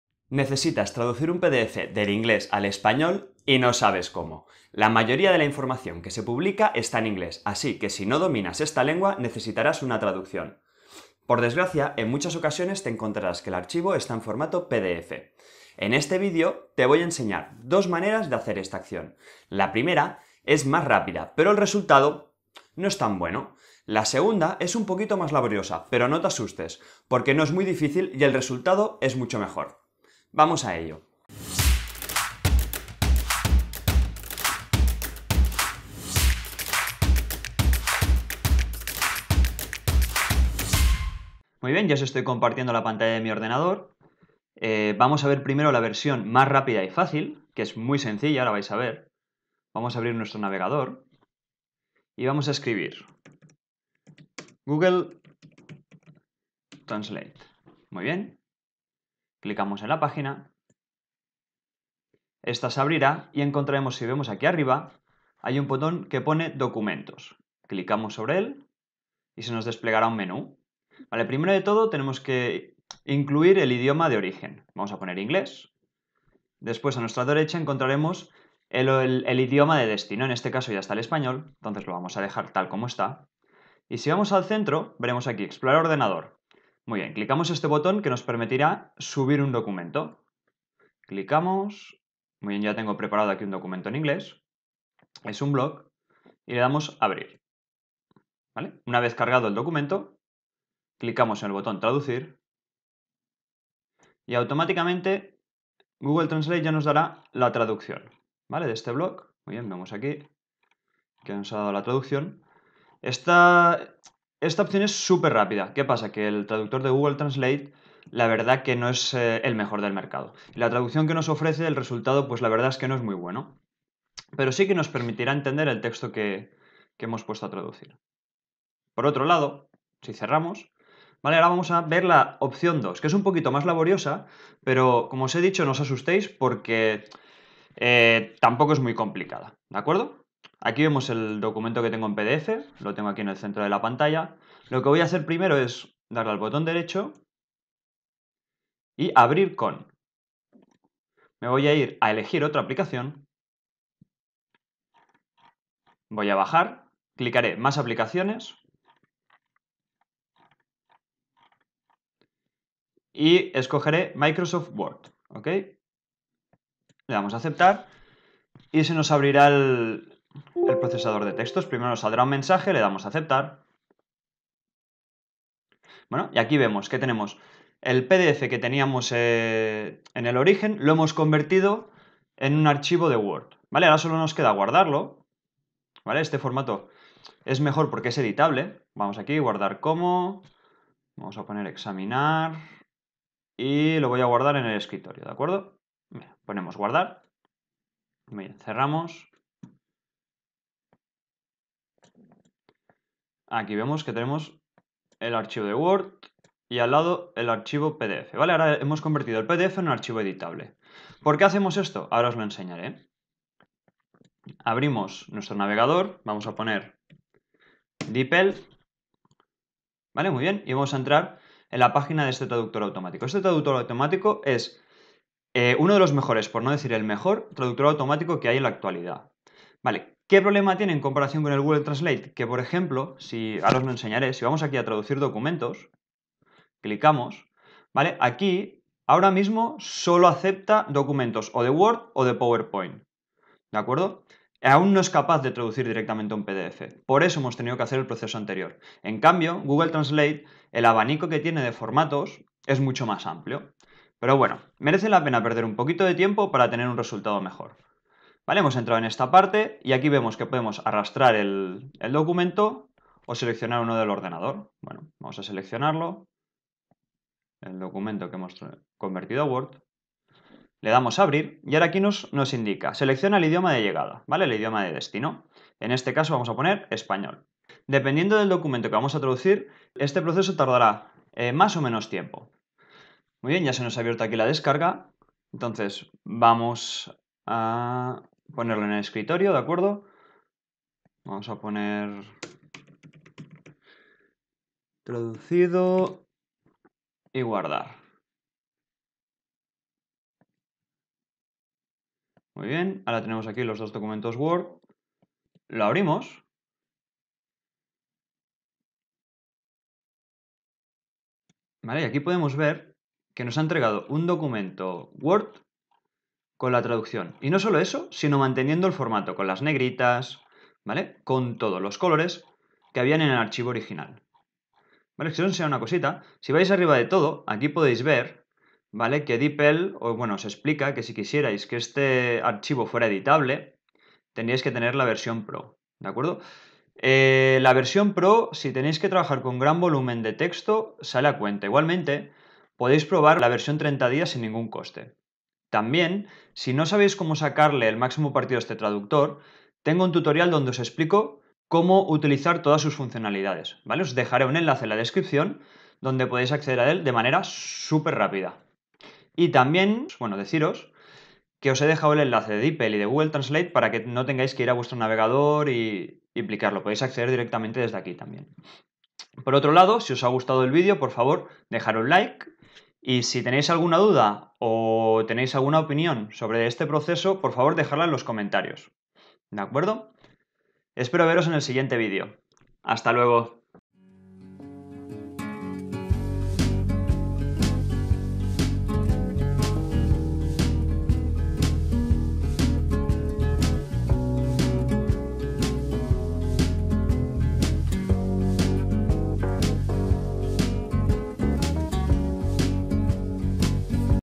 🚀 Cómo traducir un PDF del inglés al español | GRATIS
¿Necesitas traducir un PDF del inglés al español y no sabes cómo?👌 La mayoría de la información que se publica está en inglés, así que si no dominas esta lengua, necesitarás una traducción. Y, por desgracia, en muchas ocasiones te encontrarás que el archivo está en formato PDF.

En este vídeo te enseño dos métodos sencillos para traducir un PDF del inglés al español de manera gratuita.

- Método 1: podrás traducir tu PDF directamente del inglés al castellano a través de Google Traductor.

- Método 2: en este caso, primero necesitarás convertir el PDF a formato editable. Después podrás traducir el PDF del inglés al español con uno de los mejores traductores automáticos que hay actualmente: DeepL.

Si no conoces DeepL, te dejo un enlace con un tutorial sobre cómo sacarle el máximo partido a este traductor automático